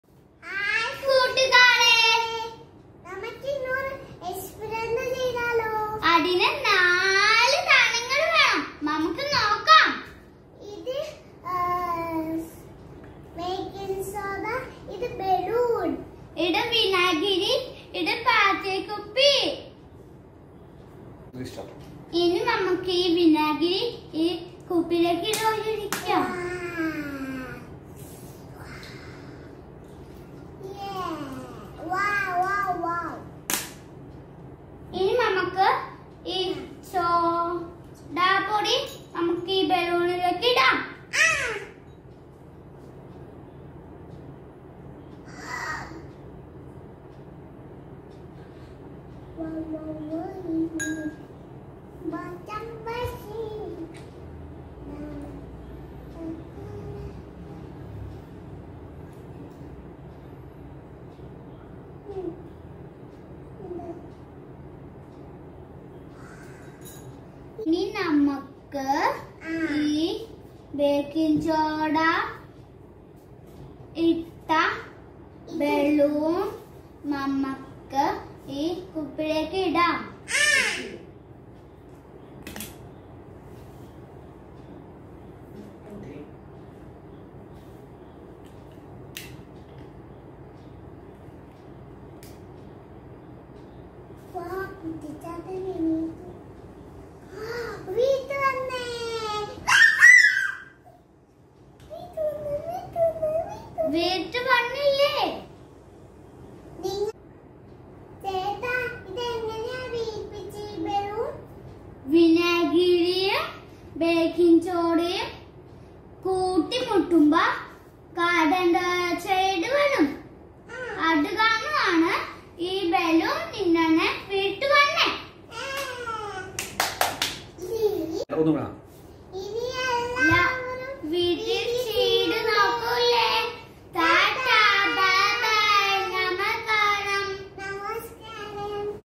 Science experiments
An experiment done by Ishan and Isha.